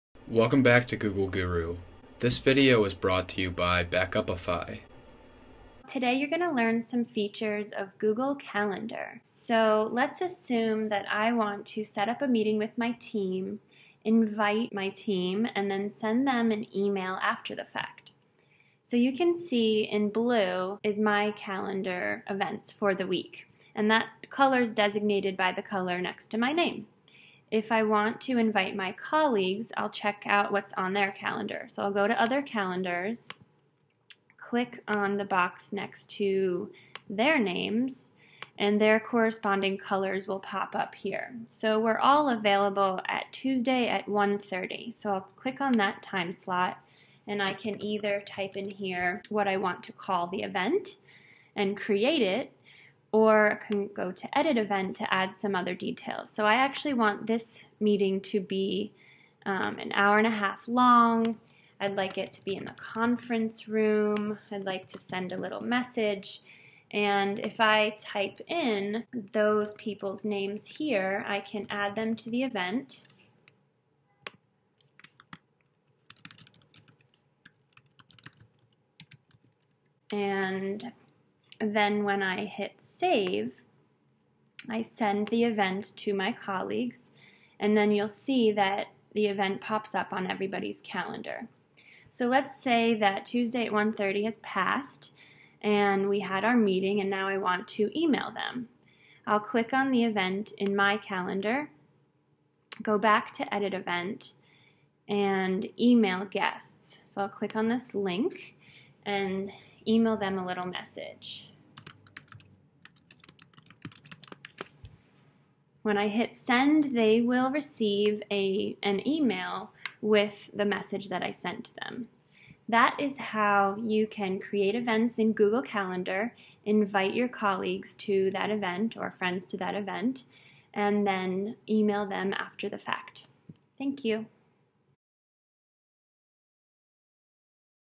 Create Events, Invite Guests and Email Attendees in Google Calendar
There are plenty of reasons why Google Calendar is our favorite tool for planning events, inviting guests and staying in touch with attendees. Check out this video for an introduction to some of the basic tools for your Calendar, some of which you should be using every day!